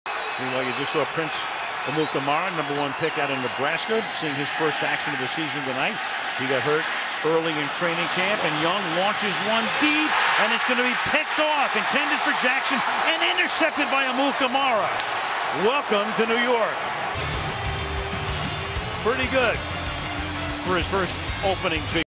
Prince Amukamara 1st Ever Pick In The NFL
Could this be a foreshadowing of what things to come from New York Giants Cornerback Prince Amukamara.